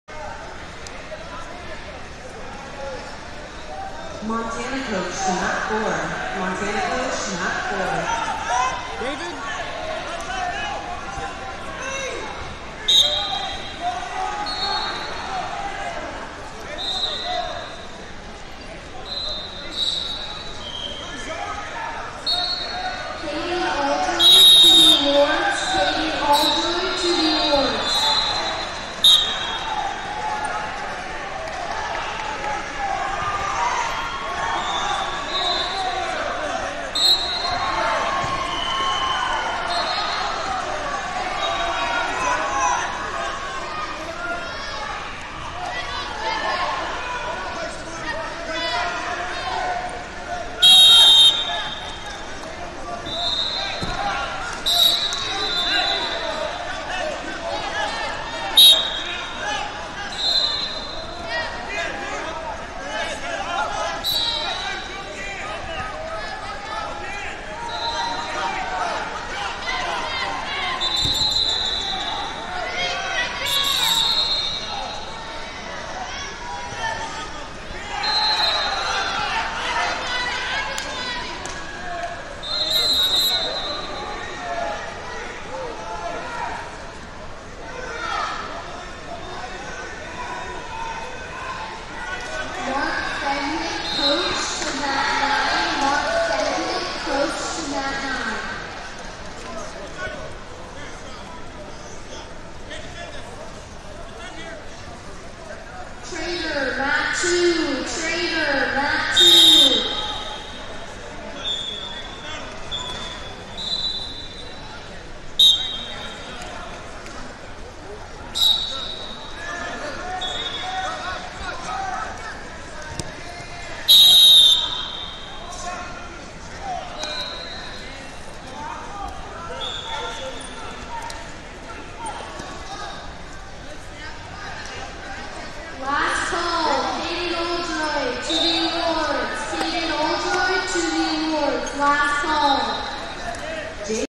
97 Lbs Cons. Round 2 - David Pina, AZ Vs Michael Kowalski, UT Ac1e
2022 Western Regional Championships - 97 Lbs Cons. Round 2 - David Pina, AZ Vs Michael Kowalski, UT Ac1e